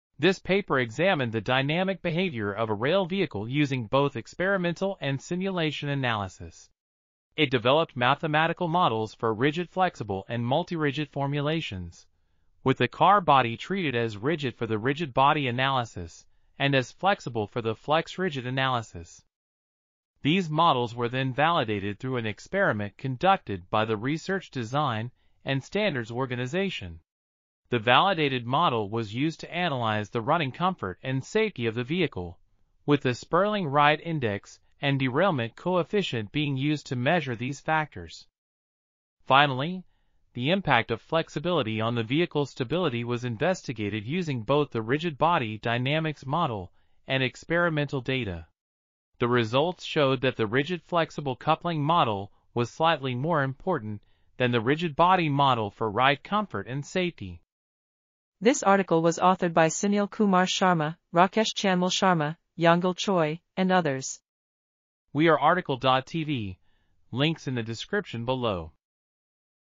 Experimental and Mathematical Study of Flexible–Rigid Rail Vehicle Riding Comfort and... | RTCL.TV
### Keywords ###

### Article Attribution ###
Title: Experimental and Mathematical Study of Flexible–Rigid Rail Vehicle Riding Comfort and Safety
Authors: Sunil Kumar Sharma, Rakesh Chandmal Sharma, Yeongil Choi ,and Jaesun Lee

### Video Timestamps ###
0:00:00 - Summary
0:00:58 - Title
0:01:04 - Outro
0:01:09 - End